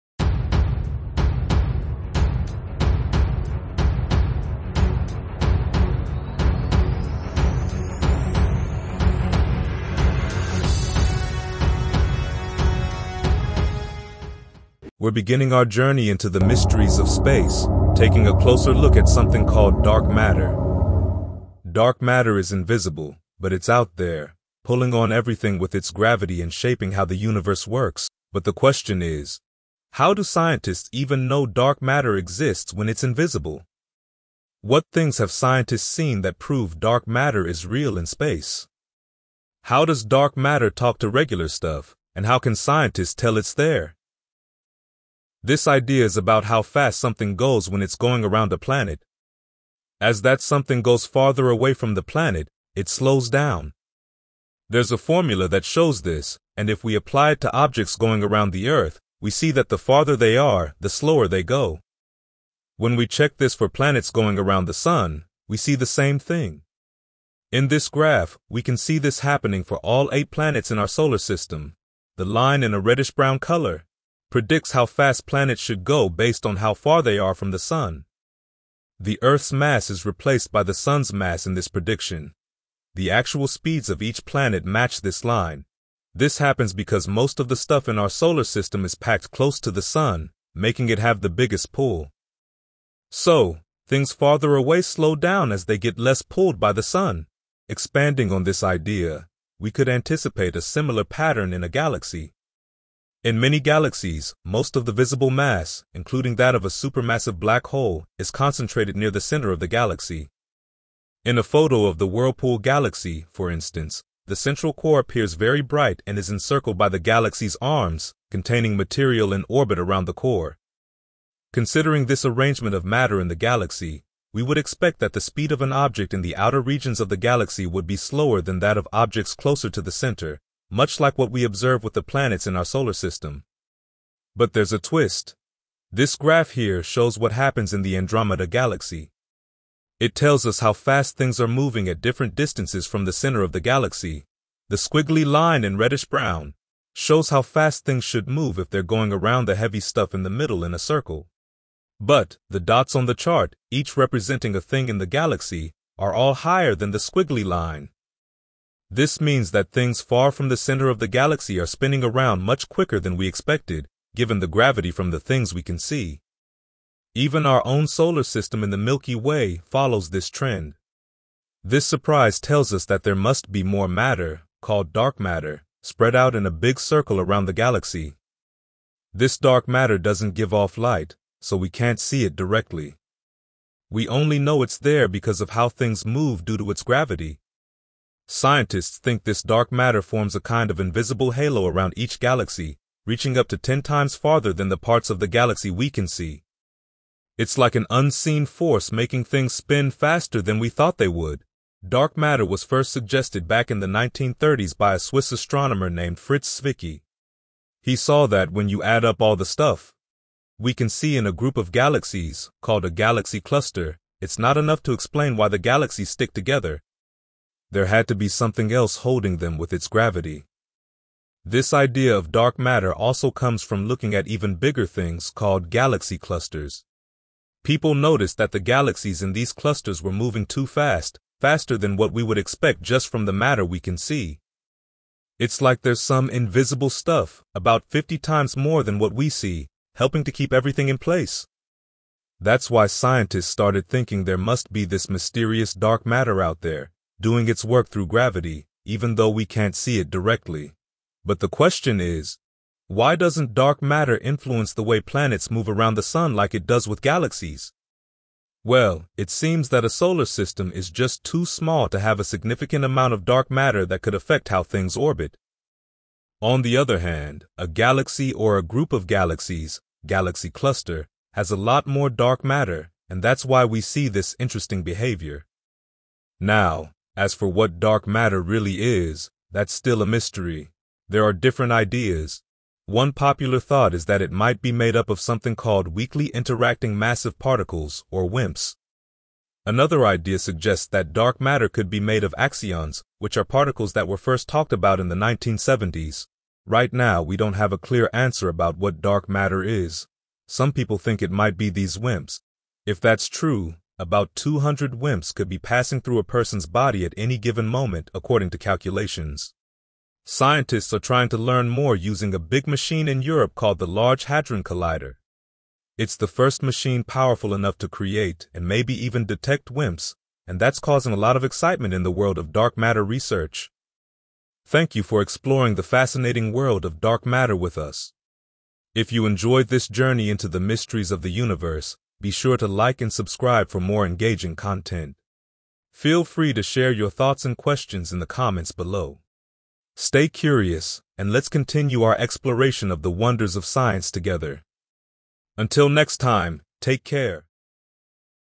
How did Scientists Discover Dark Matter if it is INVISIBLE? | Physics of DARK MATTER
What is Dark Matter? / How did Scientists discover Dark Matter? / How Dark Matter Interacts

🔭 Get to Know Dark Matter: The Cosmic Mystery
Find out about the mysterious dark matter, a hidden power that scientists can't see but know is out there. See how it influences everything in space and learn why it's such a big deal.

🌌 Take a Trip Through Space
We'll take you on a cosmic adventure with cool pictures and interesting questions. Check out the amazing things scientists have seen and why they think dark matter is real.

🤔 Curious? We Have Questions!
Let your curiosity lead the way! We're asking some cool questions about dark matter. How do scientists even know it's there? What clues tell us it exists? How does it work with the stuff we can see? Get ready to find out and let your imagination run wild!